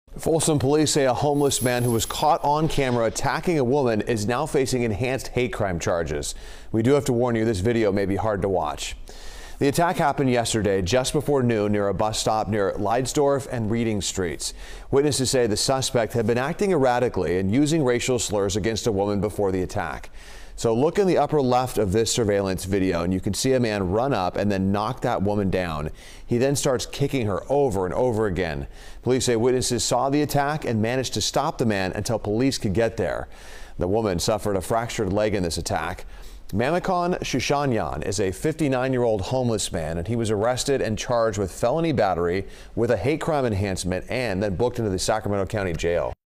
Police call attack on woman in Folsom a hate crime
A man faces a hate crime enhancement after he attacked a woman on Tuesday, according to the Folsom Police Department